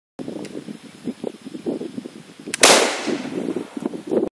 pistoluniform
Firing the doglock horse pistol, in Musketeer uniform.